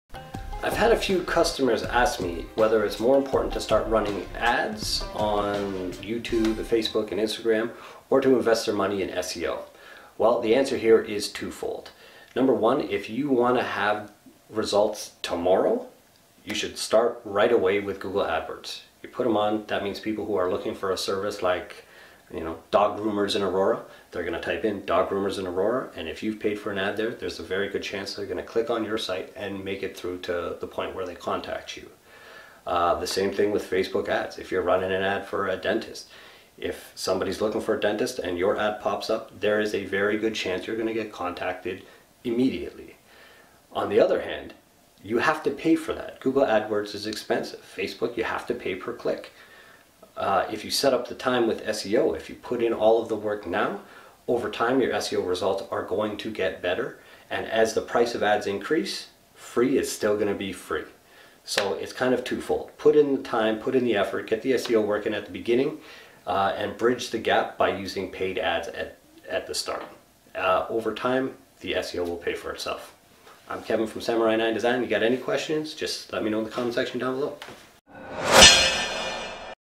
Paid Ads vs SEO...Whats Right for your business? - S9D Digital Marketing
When trying to decide the best way to find new customers for your business may people wonder the best whether they should invest in paid advertisements or Search Engine Optimization.

In a perfect world, a company would stack the deck and give themselves the absolute best chances of finding more of the right customers by investing in both.

If you have to choose only one avenue, this is how I explain things.   Social Media Advertising will work immediately but it has a cost associated and always will.

SEO, on the other hand, is paid for upfront and takes months, if not years to kick in.  However, once it starts to work, the leads become very inexpensive if not free.